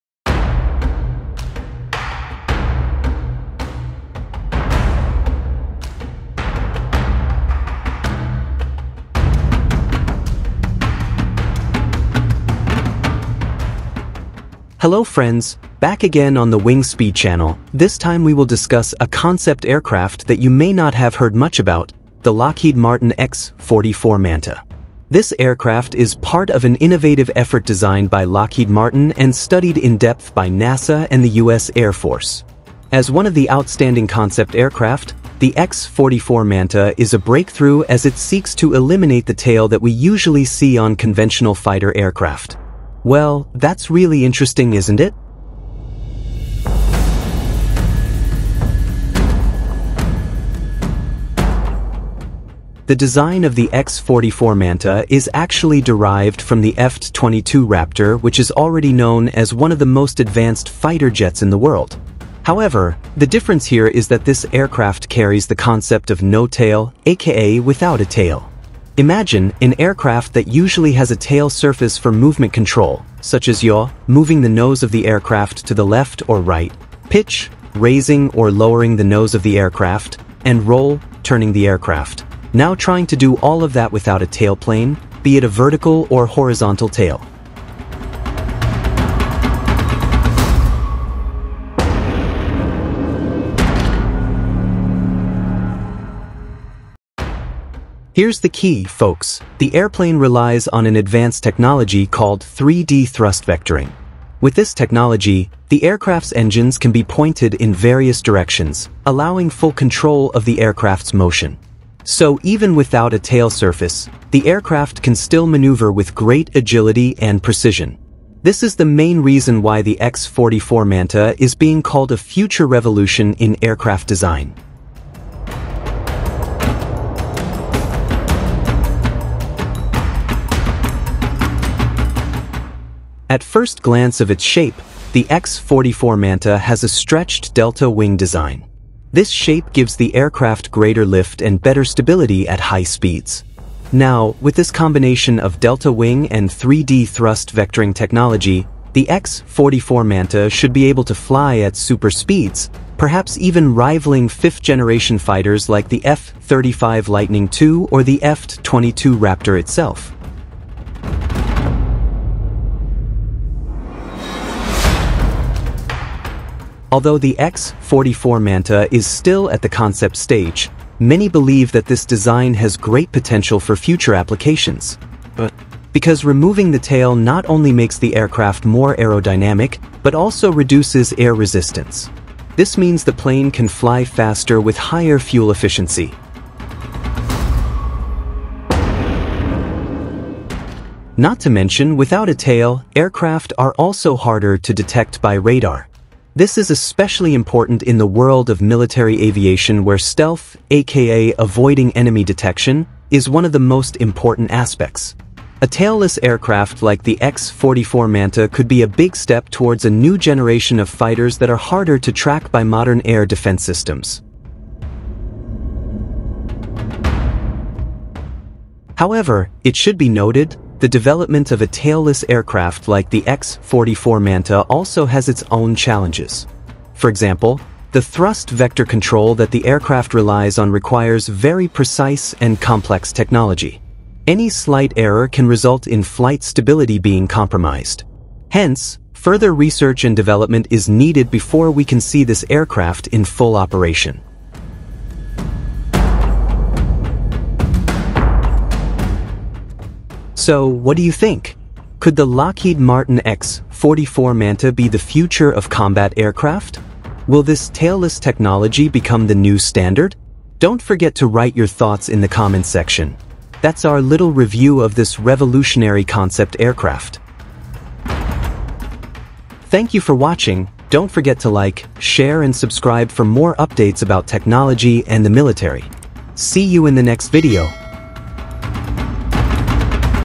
Lockheed Martin X-44 MANTA: A Revolution in Tailless Aircraft Design
Lockheed Martin dreamed of a fighter aircraft that could be as stealthy as possible without sacrificing maneuverability. The result was a sleek airframe with a remarkable characteristic: it had no tail.

The Multi-Axis No-Tail Aircraft, or X-44 MANTA, was an outstanding concept that would allow thrust vector control like no other aircraft before, but it proved to be too avant-garde for its time.